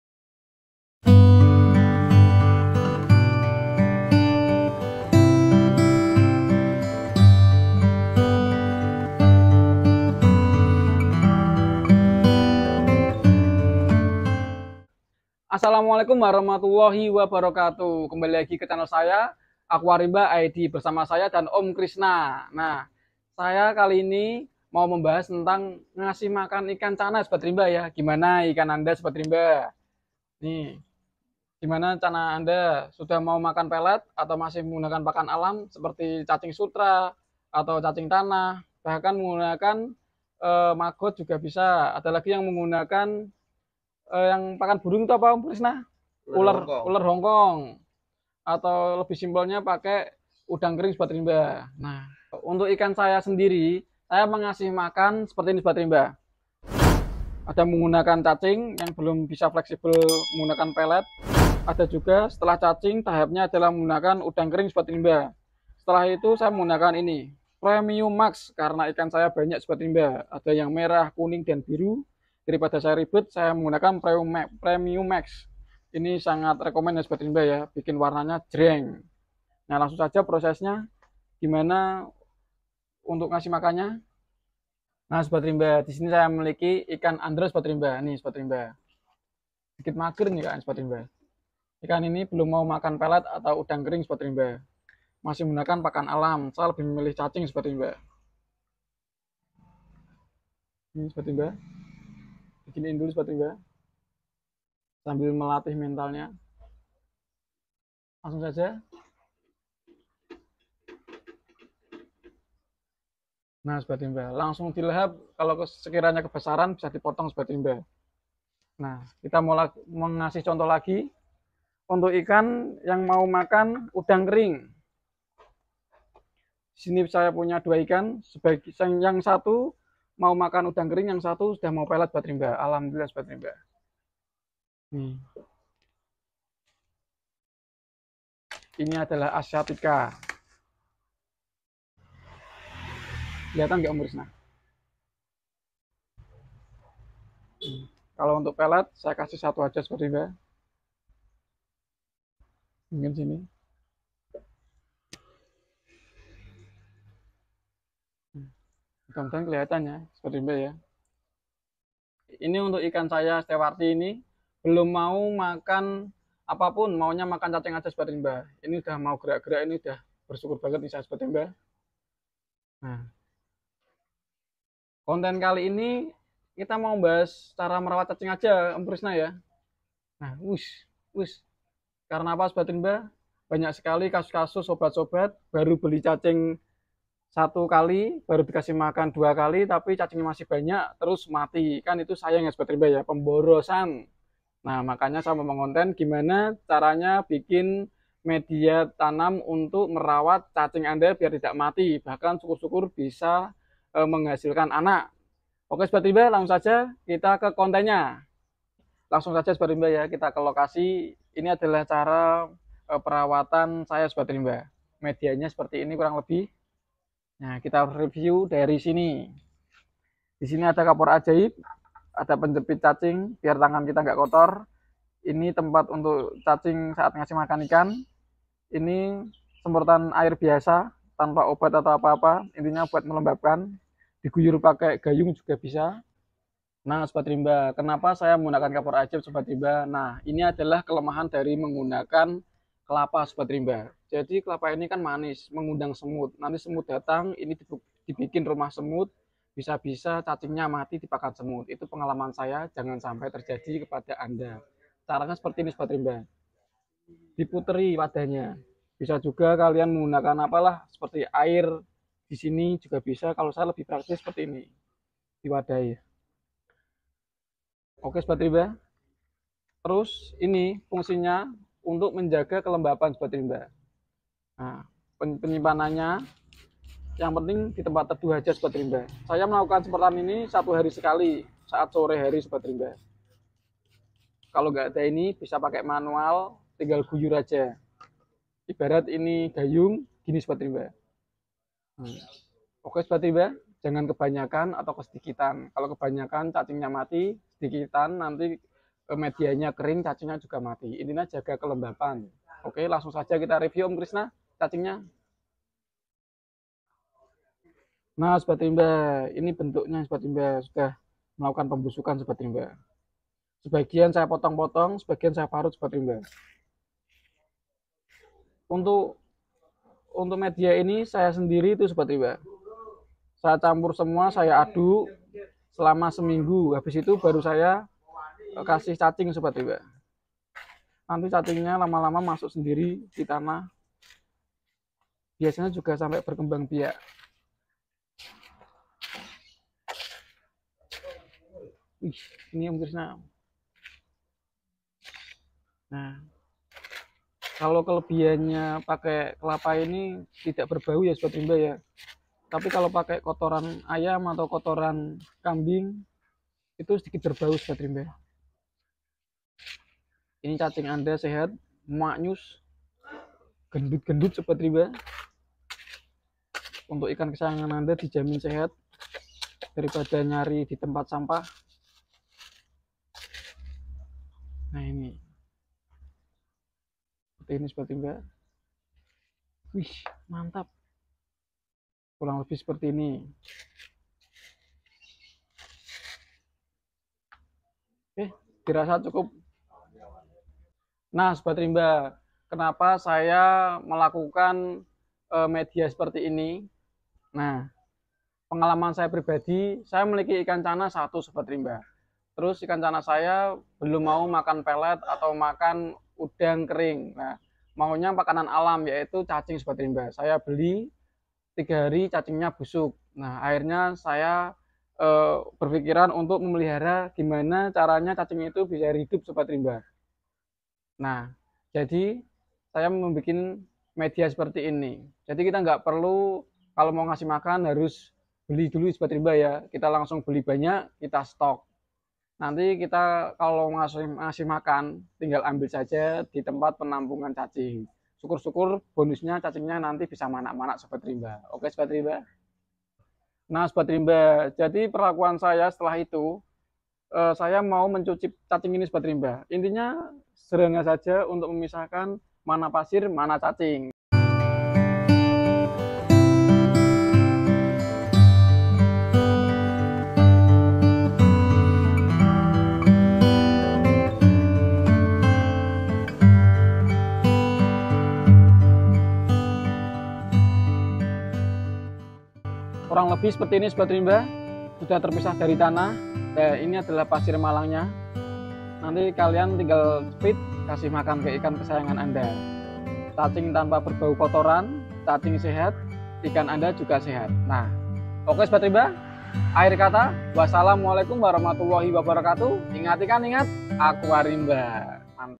MEMELIHARA CACING TANAH UNTUK MAKAN IKAN CHANNA
Pada vidio kali ini @aquarimbaid7284 membagikan sebuah vidio cara memelihara cacing tanah untuk makan ikan channa